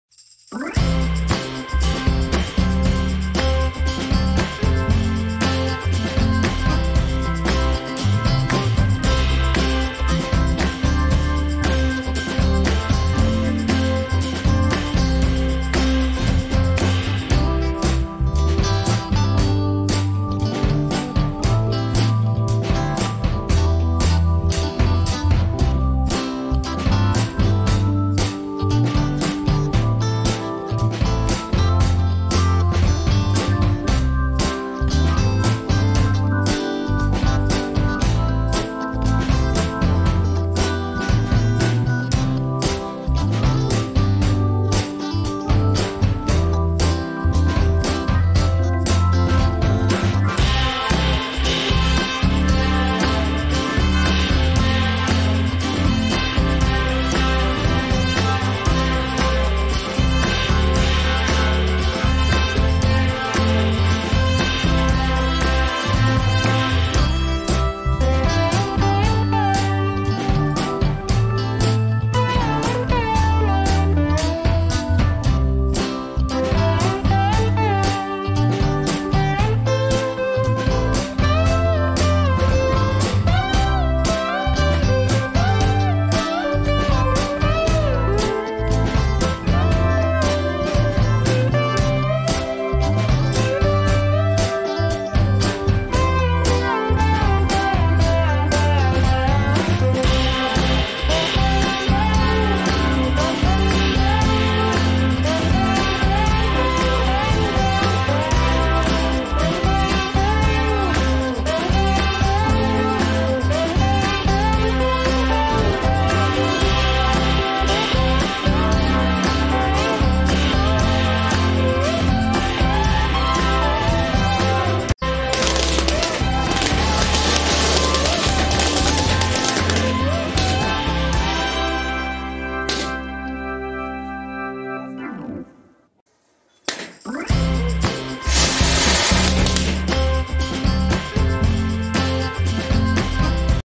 FUN POPPING LOTS OF BALLOONS PART 69!#69🎈🎈🎈
Hello guys,
Today we are inflating lots of Balloons and Popping them using our Hands!
Hope you enjoy our show!